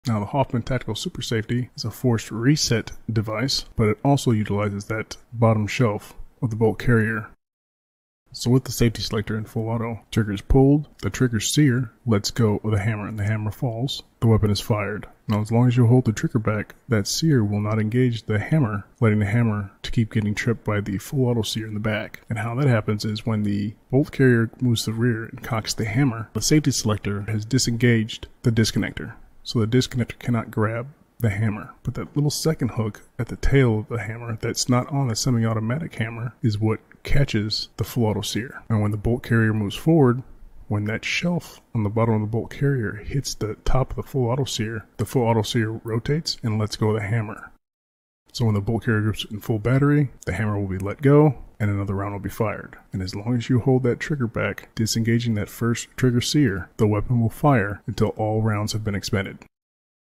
How An Auto Sear Works In An AR15
Understanding AR-15 full auto and semi-auto mechanics.